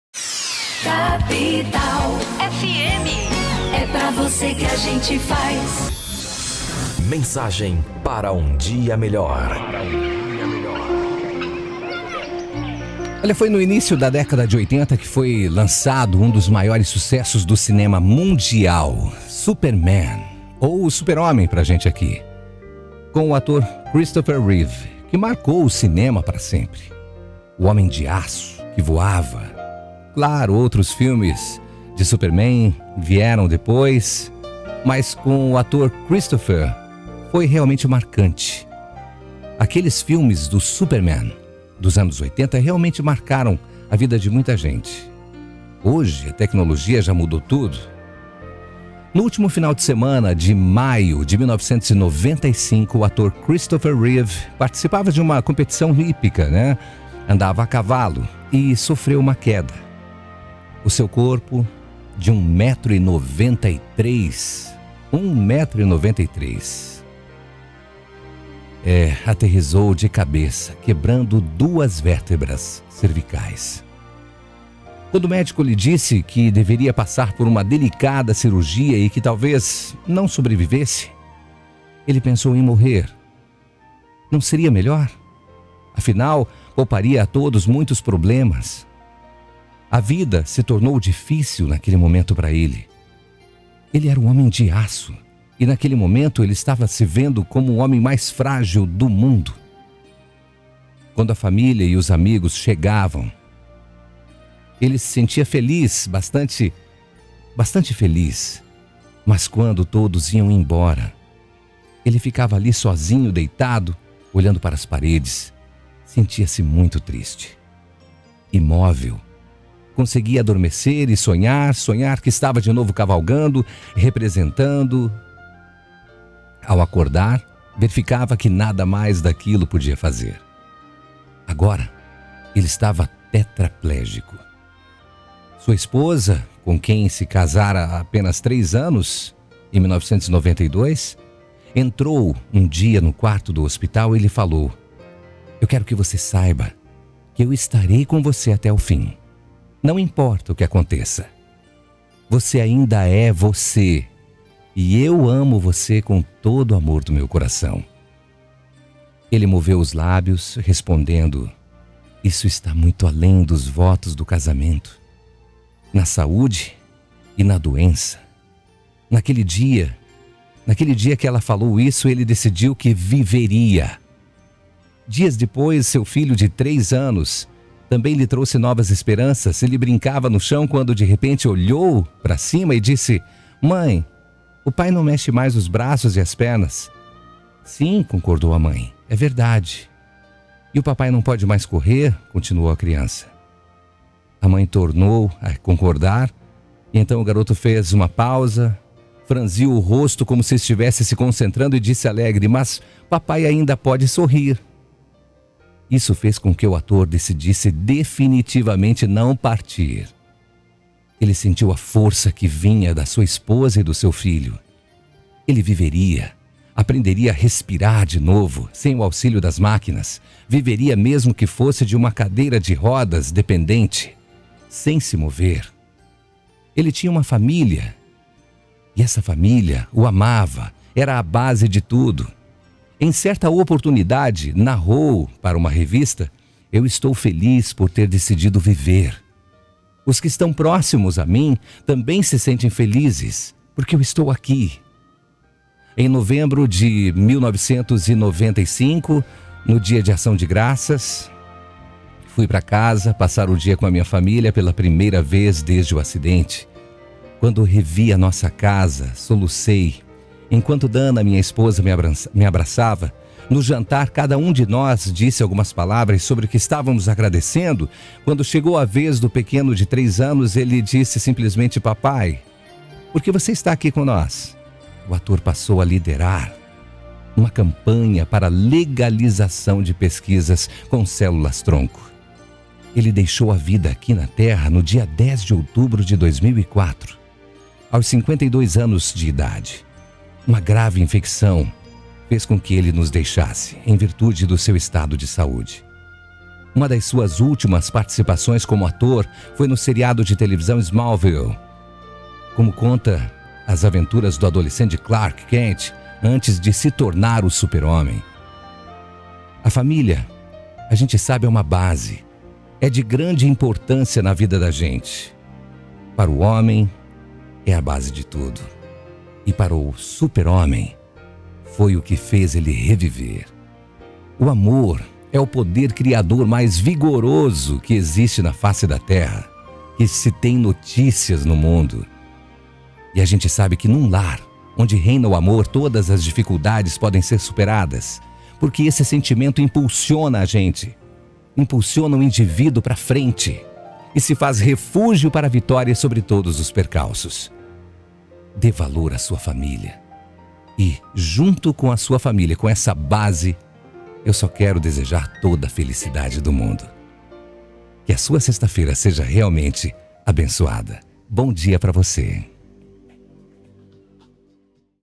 Mensagem para o Dia Melhor - Familia a base de tudo
Família, a base de tudo
Foi no inicio da década de 80 que foi lançado um dos maiores sucessos no cinema mundial Superman ou Super homem, com o ator Christipher Reeve que marcou o cinema para sempre, o homem de aço que voava, claro outros filmes de superman vieram depois mais com o ator Christipher foi marcante. No último fim de semana de maio de 1995, o ator Christopher Reeve, participava de uma competição hípica e sofreu uma queda. Seu corpo, de um metro e noventa e três centímetros e noventa e sete quilos, aterrissou de cabeça, quebrando duas vértebras cervicais. Quando o médico lhe disse que deveria passar por uma delicada cirurgia e que talvez não sobrevivesse, ele pensou em morrer. Não seria melhor? Afinal, pouparia a todos muitos problemas. A vida se tornou difícil. Quando a família e os amigos chegavam, ele se sentia feliz. Mas quando todos iam embora e ele ficava ali, sozinho, deitado, olhando para as paredes, sentia-se muito triste. Imóvel, conseguia adormecer e sonhar. Sonhar que estava de novo cavalgando, representando. Ao acordar, verificava que nada mais daquilo poderia fazer, agora ele estava tetraplégico. Sua esposa, com quem se casara há apenas três anos, entrou um dia no quarto do hospital e lhe falou: Quero que você saiba que estarei com você até o fim, não importa o que aconteça. Você ainda é você e eu o amo com todo meu amor. Ele moveu os lábios, respondendo: Isso está muito além dos votos do casamento: na saúde e na doença. Naquele dia ele decidiu que viveria. Dias depois, seu filho de três anos também lhe trouxe novas esperanças. Ele brincava no chão quando, de repente, olhou para cima e disse: Mãe, o papai não mexe mais os braços e as pernas. Sim, concordou, a mãe. É verdade. E o papai não pode mais correr, continuou a criança. A mãe tornou a concordar. Então, o garoto fez uma pausa, franziu o rosto como se estivesse se concentrando e disse alegre: Mas papai ainda pode sorrir. Isso fez com que o ator decidisse definitivamente não partir ele sentiu a força que vinha de sua esposa e de seu filho. Ele viveria. Aprenderia a respirar sem o auxílio da máquina. Viveria, mesmo que fosse em uma cadeira de rodas, sem se mover. Ele tinha uma família. E esta família o amava era a base de tudo. Em certa oportunidade, narrou para uma revista: Estou feliz por ter decidido viver. Os que estão próximos a mim também se sentem felizes.
Em novembro de um mil novecentos e noventa e cinco, no dia de ação de graças, fui para casa passar o dia com minha família, pela primeira vez desde o acidente. Quando revi nossa casa, solucei, enquanto Dana, minha esposa, me abraçava. No jantar, cada um de nós disse algumas palavras sobre o que estávamos agradecendo. Quando chegou a vez do pequeno de três anos, ele disse simplesmente: Papai, porque está aqui com nós. O ator passou a liderar uma campanha pela legalização de pesquisas com células-tronco. Ele deixou a vida aqui na terra no dia 10 de outubro de 2004 aos 52 anos de uma grave infecção, em virtude do seu estado de saúde. Uma de suas últimas participações como ator foi no seriado de televisão "Smallville", que conta as aventuras do adolescente Clark Kent antes de se tornar o Super-Homem. * A família é de grande importância para o homem é a base de tudo. O amor é o poder criador mais vigoroso de que se tem notícias no mundo.
Num lar, onde reina o amor, todas as dificuldades podem ser superadas, porque este sentimento impulsiona o indivíduo para a frente e se faz refúgio para a vitória sobre todos os percalços.